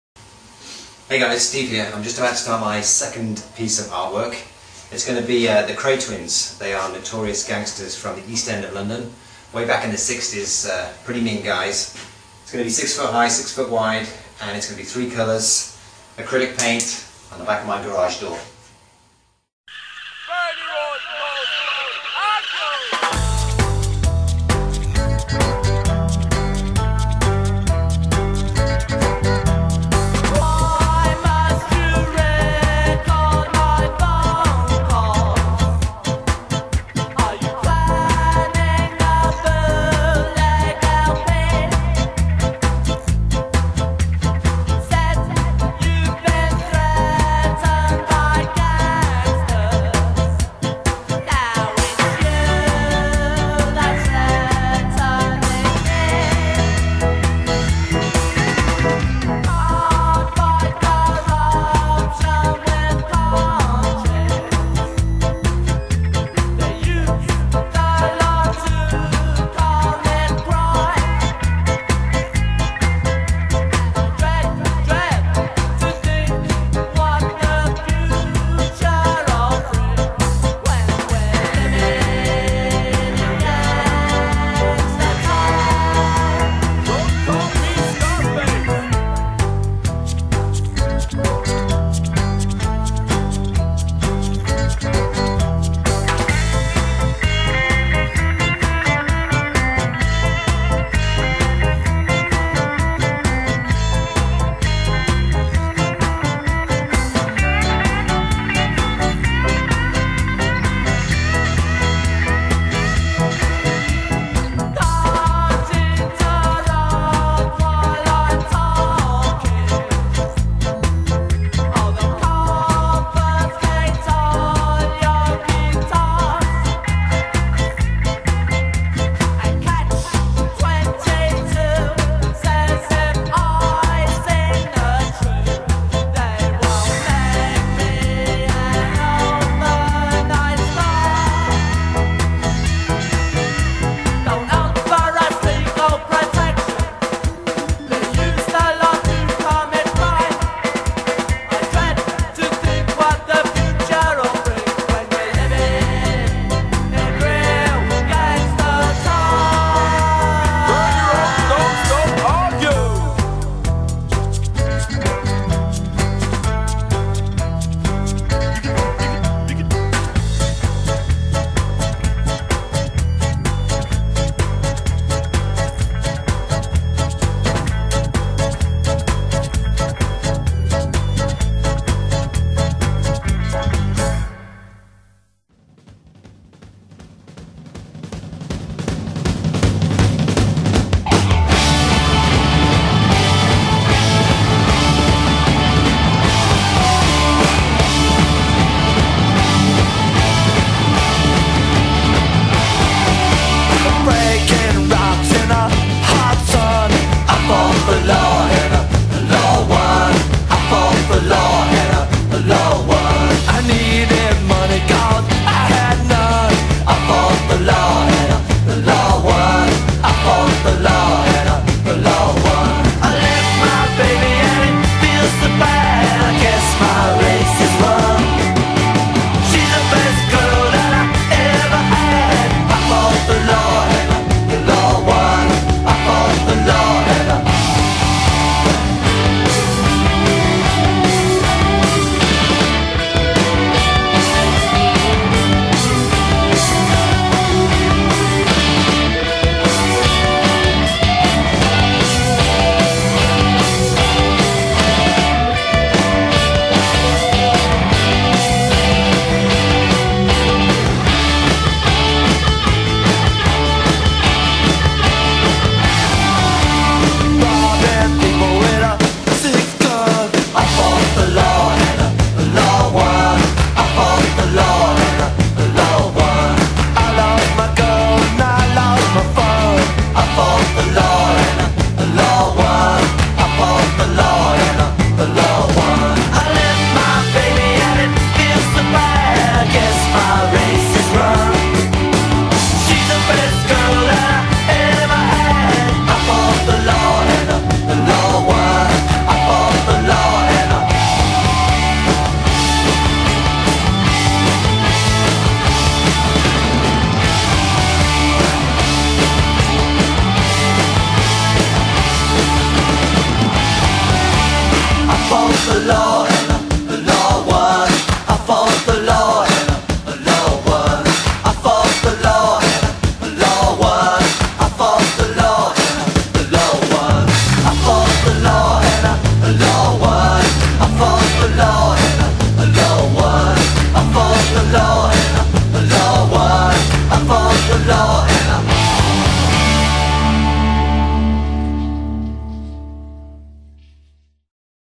The Kray Twins Art Project.wmv
The Kray Twins "Legend". A time lapse video of my art project "The Kray Twins" a pair of notorious London, East End gangsters from the 60's. The image is 6' high, 5' wide